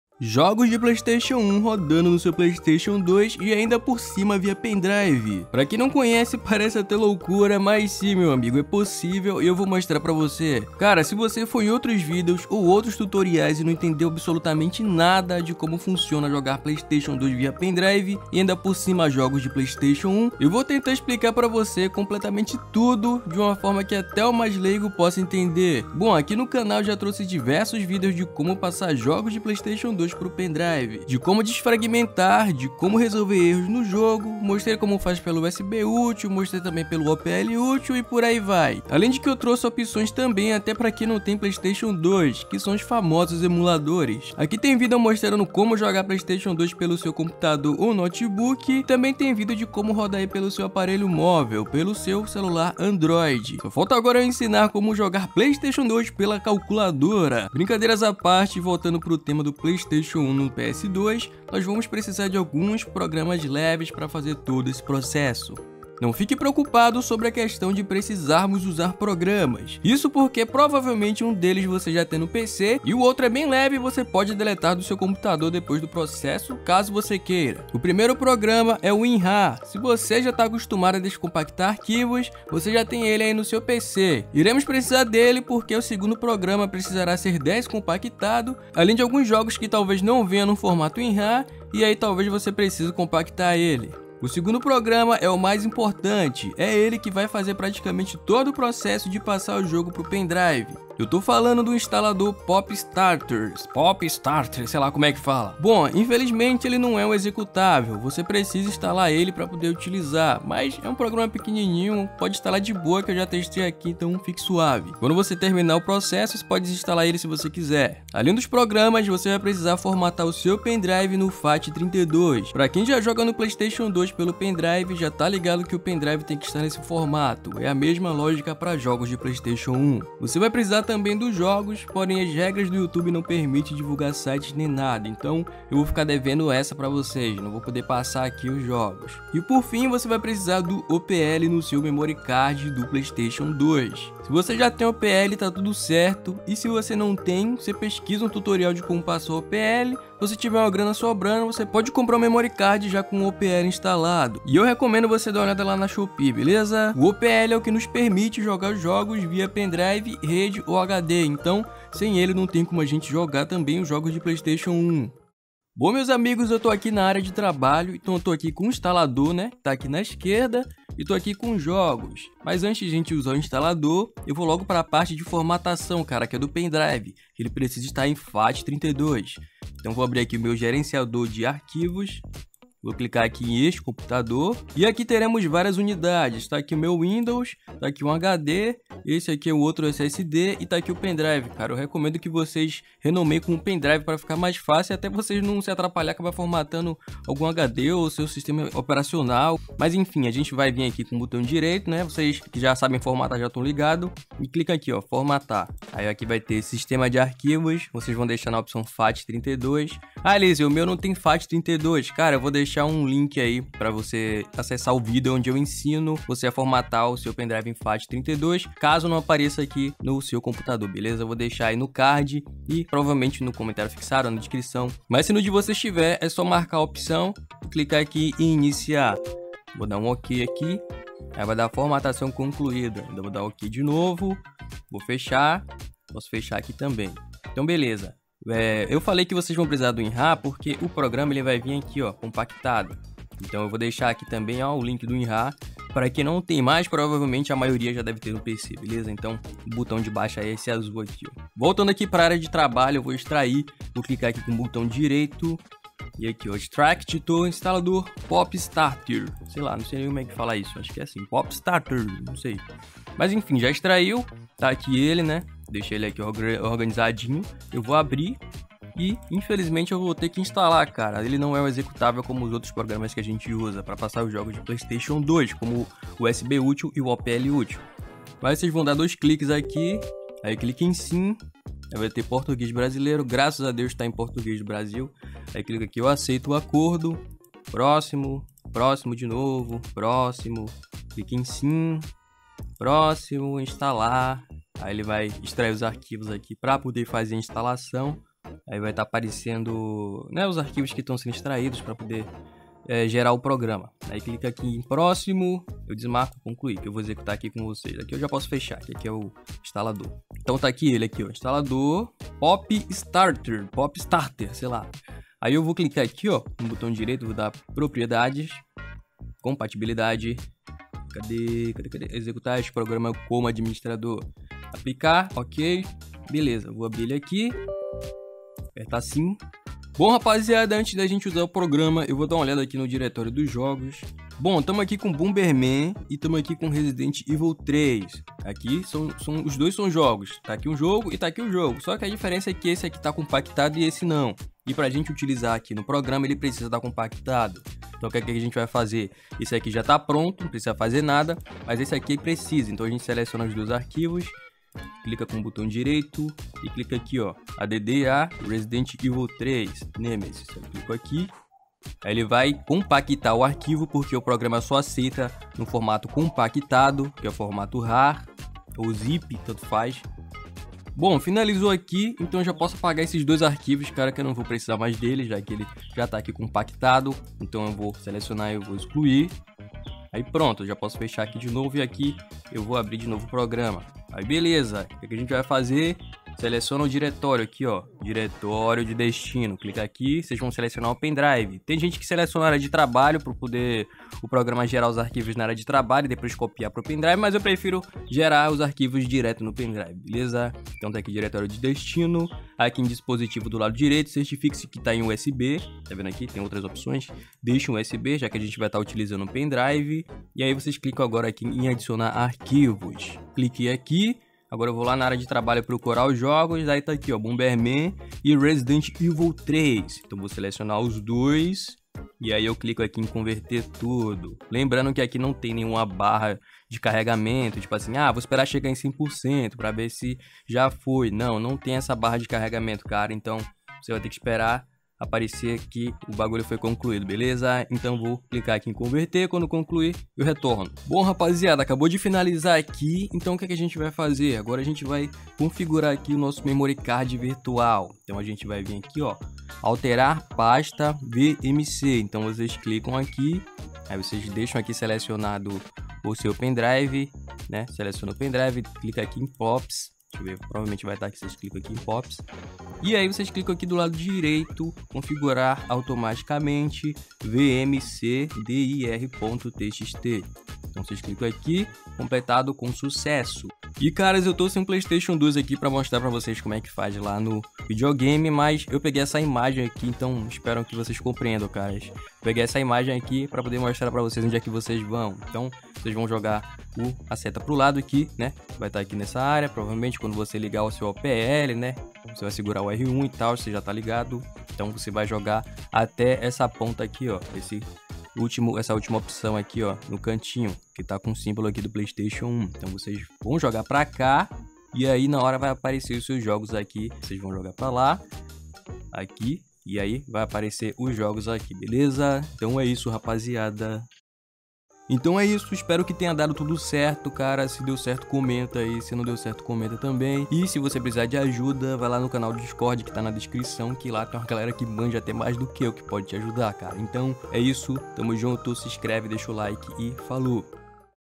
COMO RODAR JOGOS DE PS1 NO PS2 VIA USB (Pendrive e HD) POPSTARTER 2023 no OPL
✅ Aprenda a JOGAR PS1 através do seu PS2 via USB utilizando um Pendrive ou HD, através do OPL, utilizando o POPSTARTER ATUALIZADO em 2023, seu Playstation 2 rodando jogos de PS1, nesse vídeo aprenda como faz para rodar os jogos. 🕹🎮👾

00:00 - Intro Importante
01:00 - Oque vamos precisar?
02:45 - Tutorial
07:21 - Memory card
07:53 - Acessando os Jogos